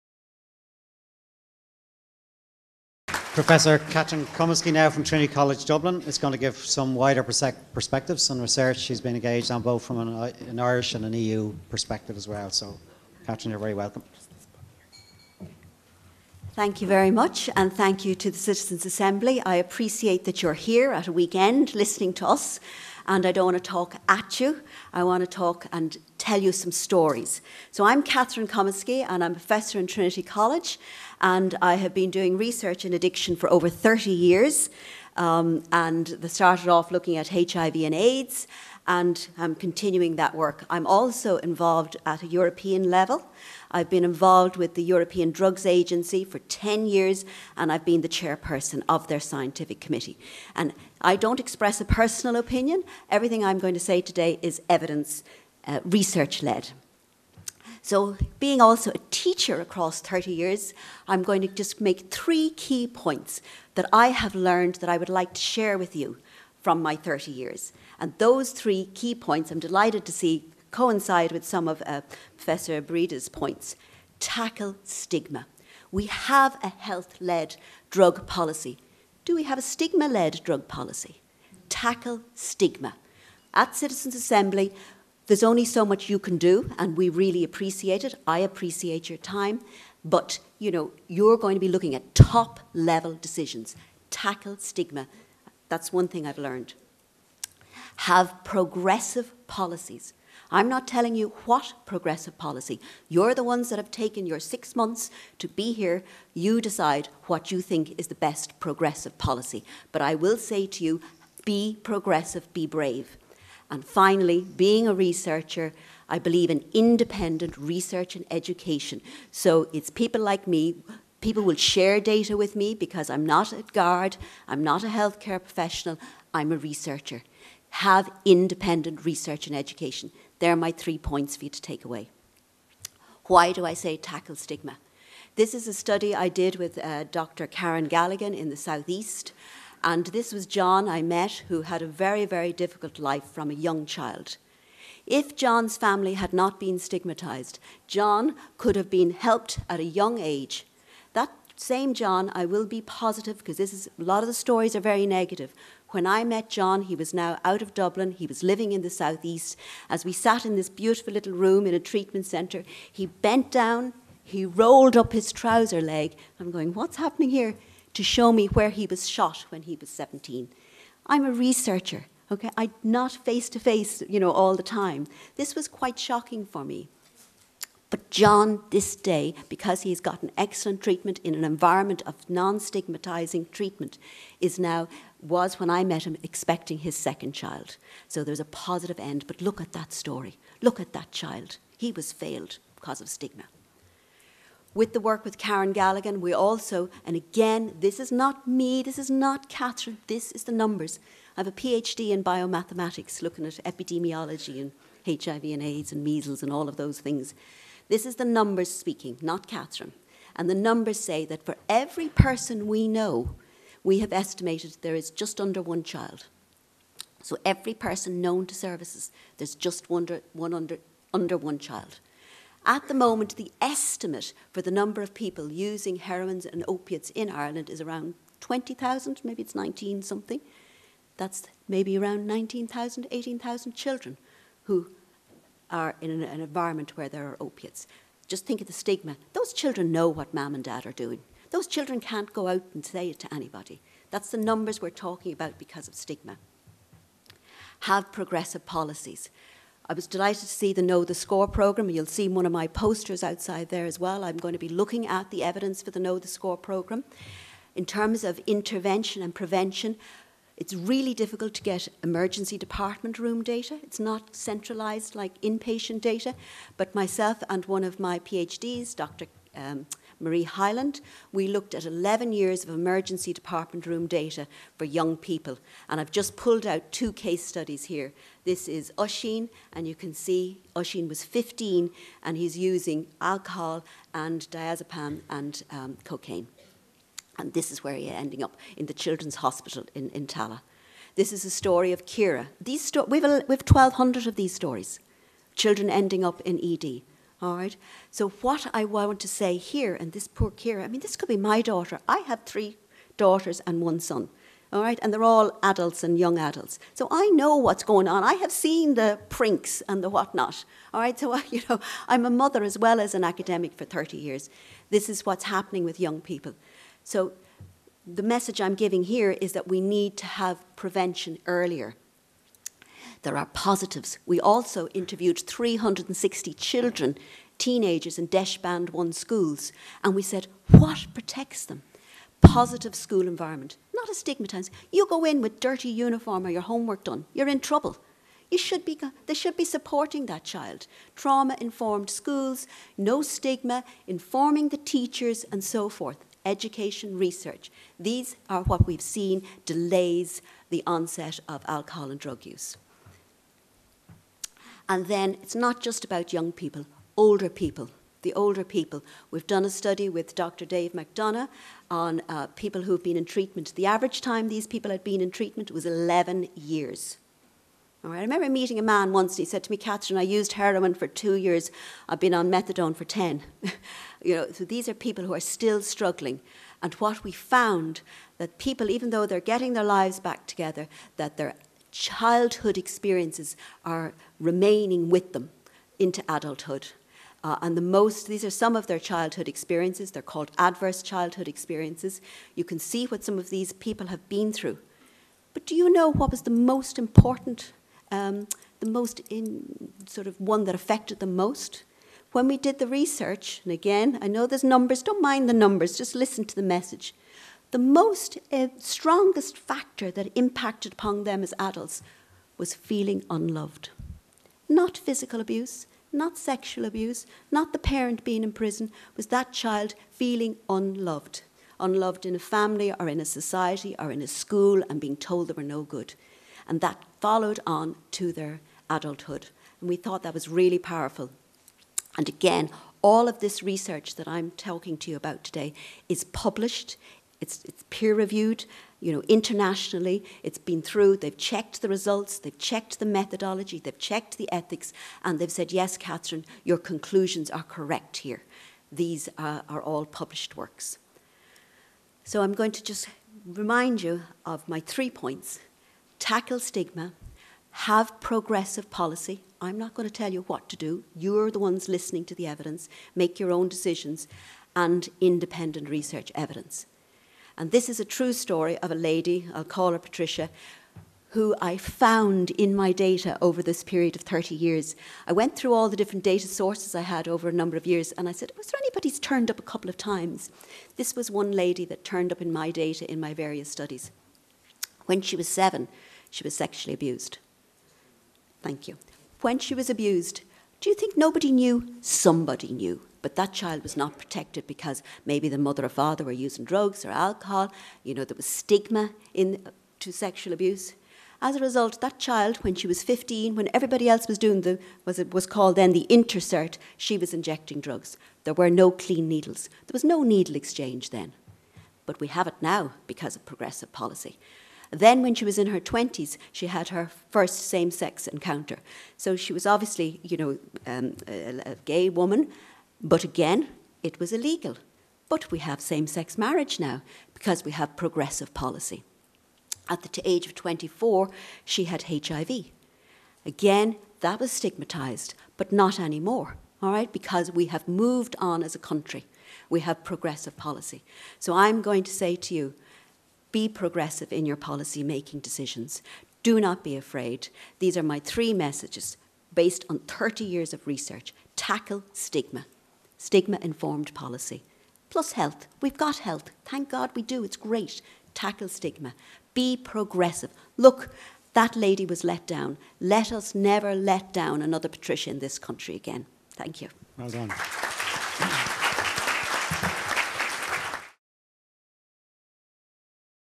CADrugsUse - Sat 30 Sept - Prof Catherine Comiskey
Research and evaluation
Prof. Catherine Comiskey, Professor in Healthcare Modelling and Statistics, Trinity College Dublin